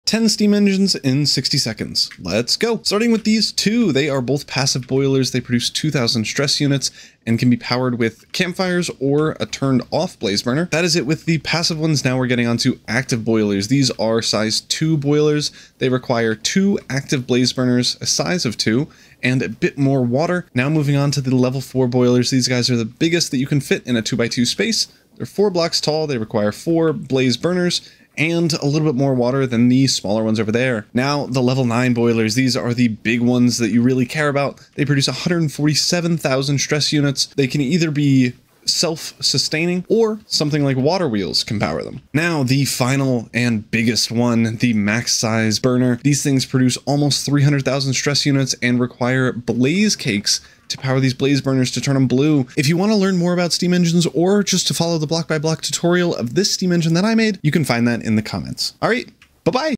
10 Steam Engines in 60 Seconds
Do you want to learn about steam engines? Well you've come to the right place! See my video in the pinned comment to learn more about steam engines and to follow the tutorial I made.